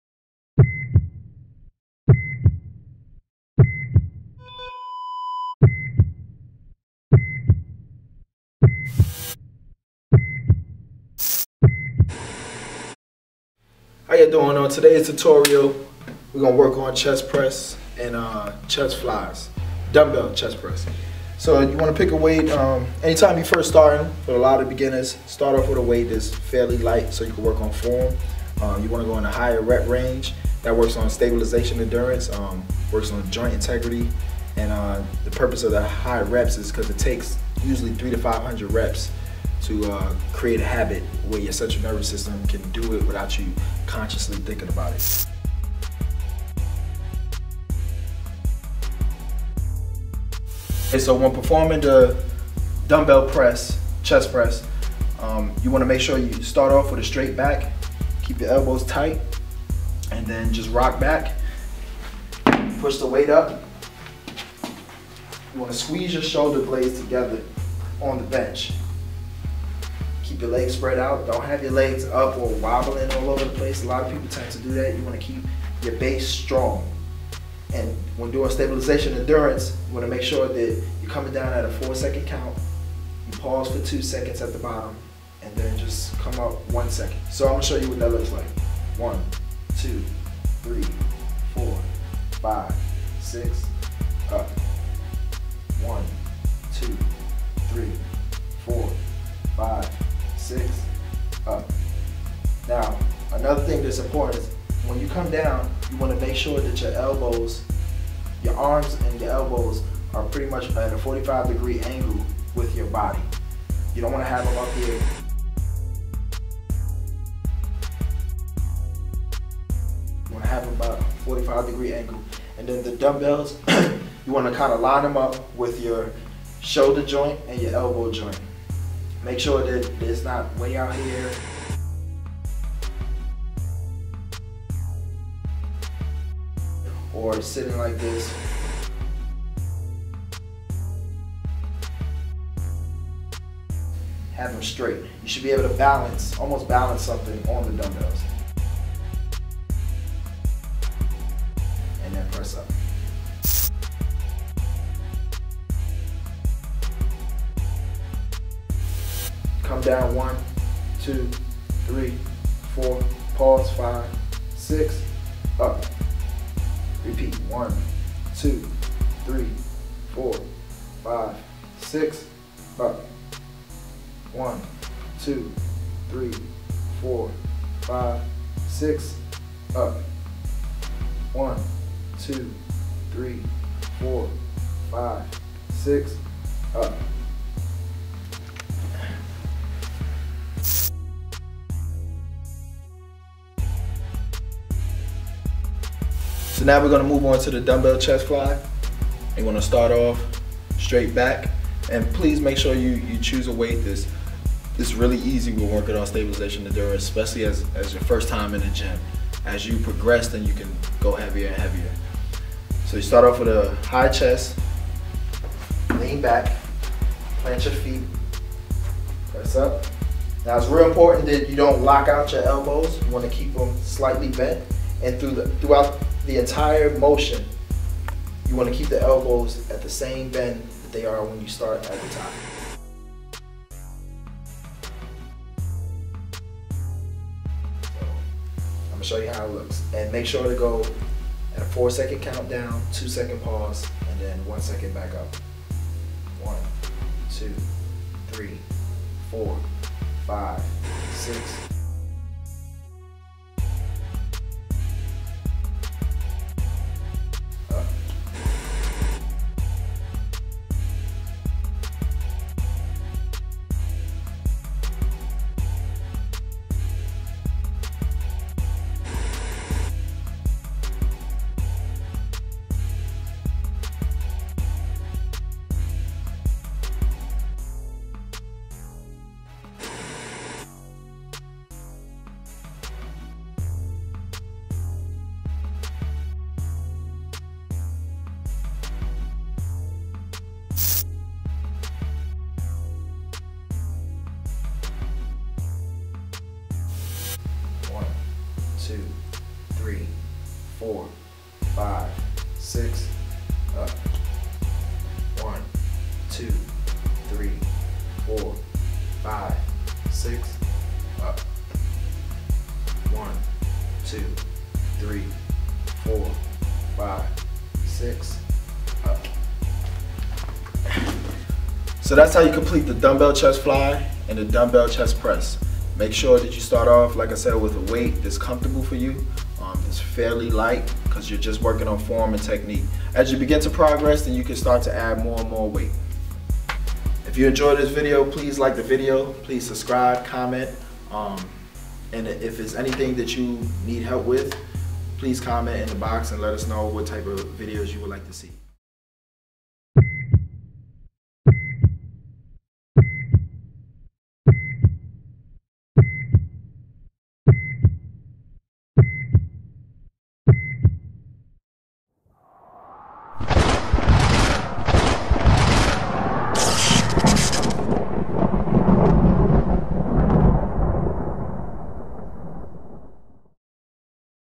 Dreambodyz Dumbbell Chest Press/Fly Tutorial
Dreambodyz CEO & Personal Trainer Strict Form gives an in-depth tutorial on how to correctly execute dumbbell chest press and dumbbell chest fly weight exercises.